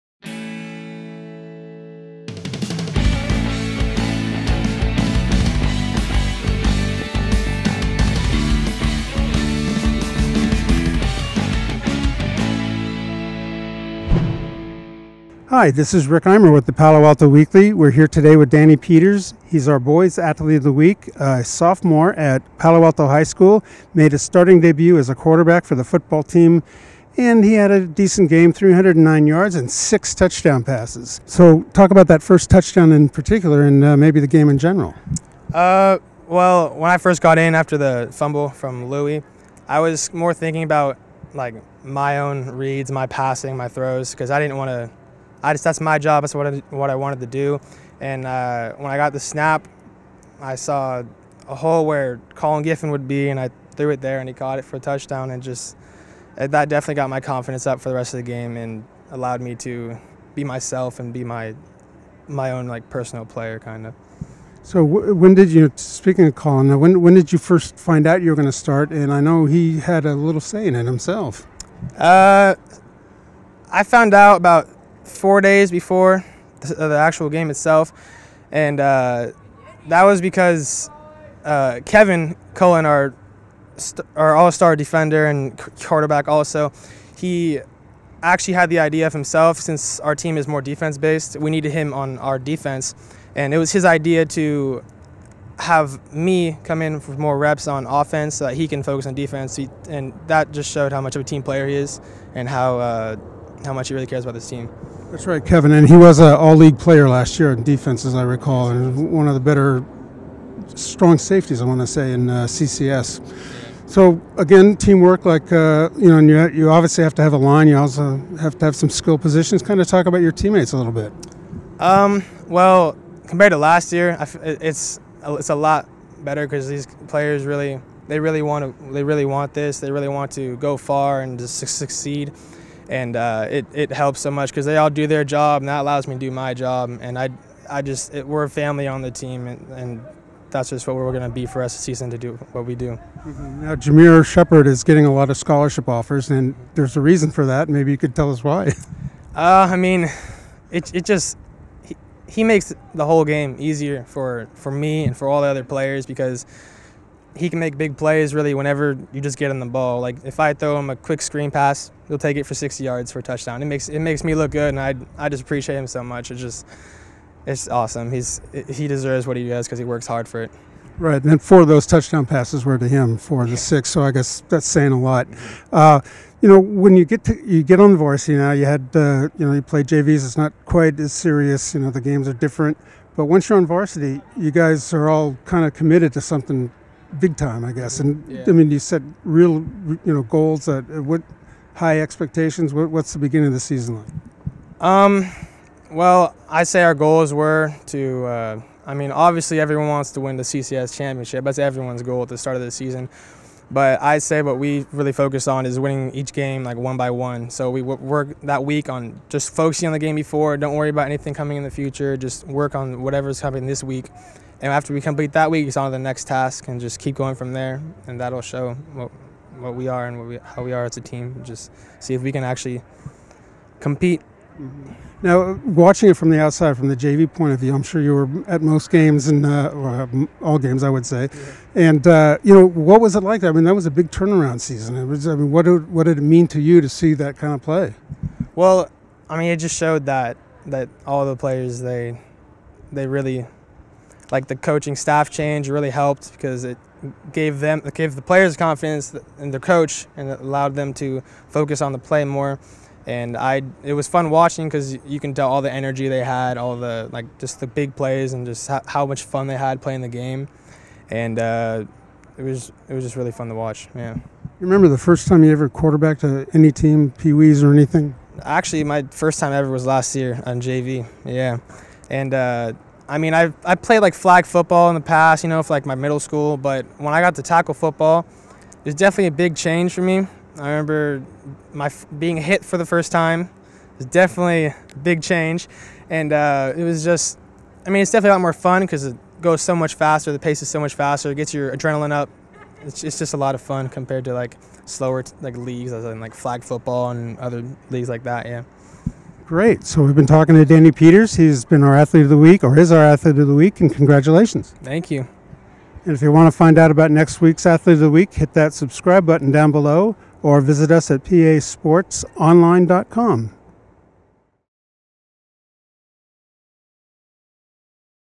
Danny Peters. Athlete of the Week
Palo Alto Weekly sports editor Rick Eymer touches base with Palo Alto High football’s quarterback Danny Peters. The sophomore talks about his starting debut and how it came about, plus how his receivers and linemen made it easy for him to throw for six touchdowns. He also talks about how watching the success of last year's team helped him step into his new position. Still photos by Karen Ambrose Hickey. Video by Palo Alto Online.